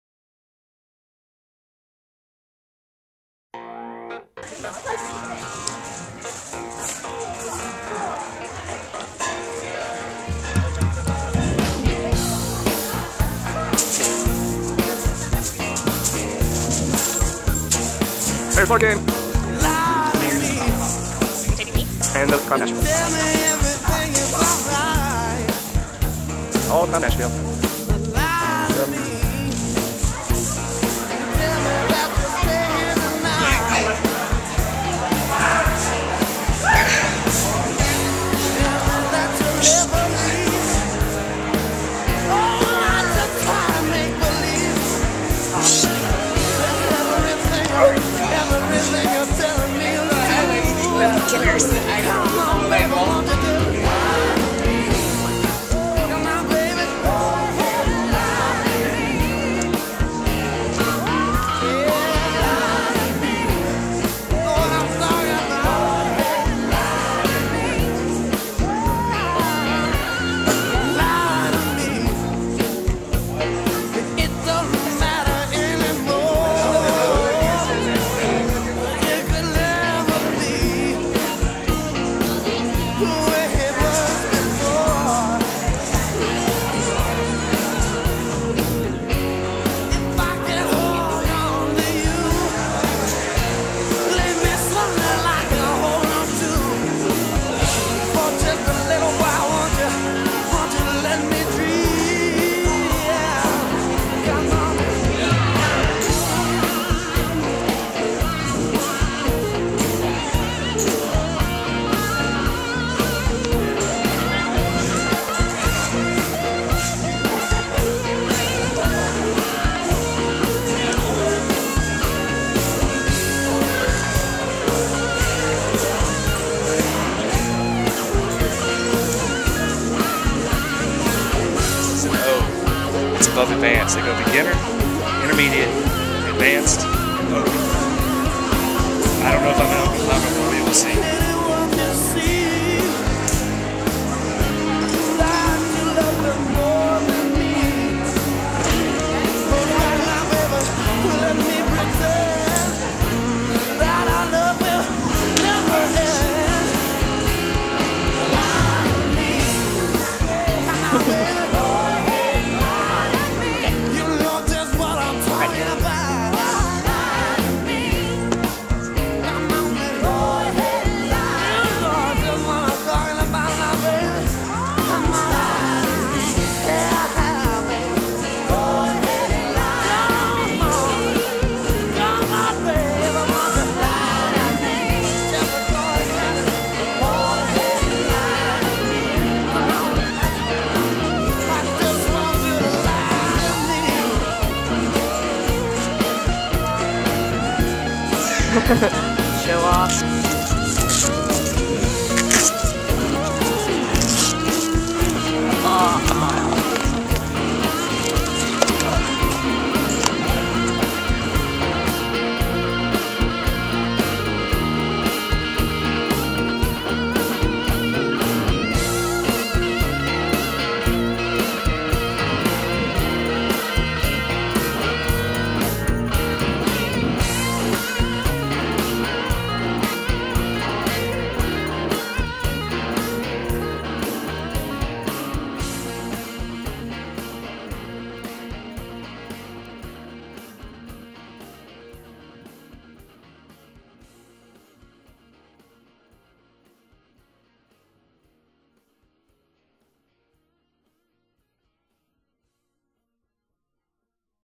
Larken & Matthew-Fun Times at Climb Nashville
Me and Matthew at Climb Nashville.